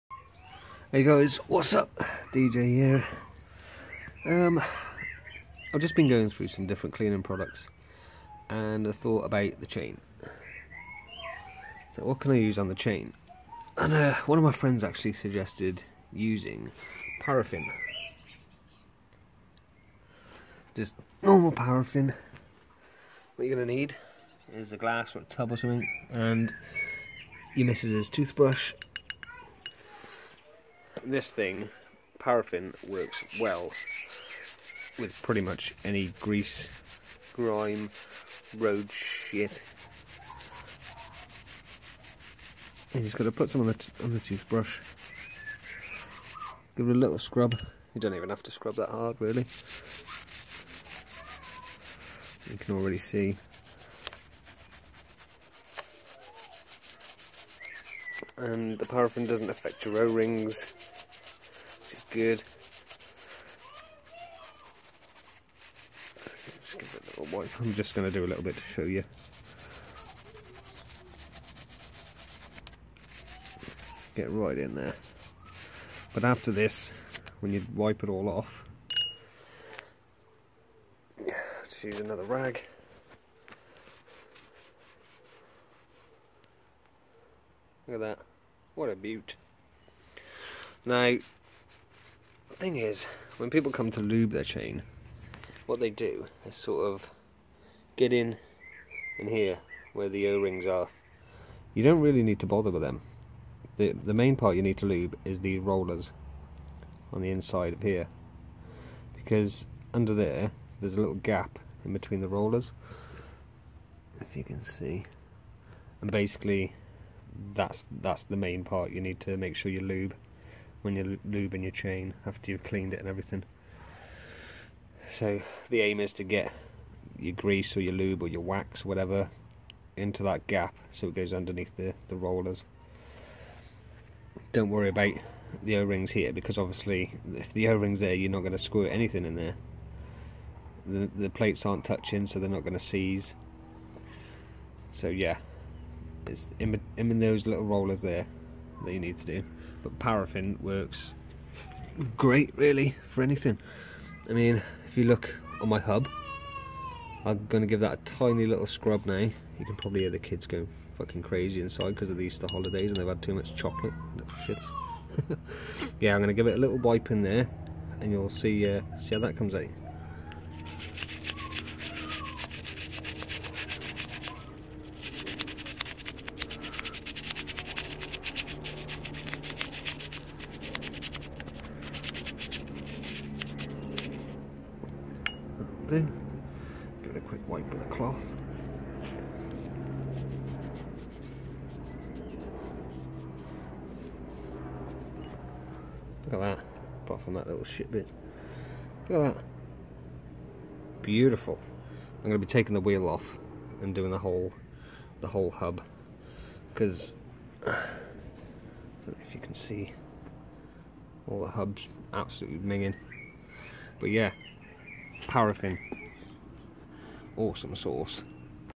EASY PEASY CHAIN/BIKE CLEANING NO EFFORT!!
What im using to get rid of dirt/oil/grime from the bike! Works amazingly, doesnt perish o/xring chains and cheap!!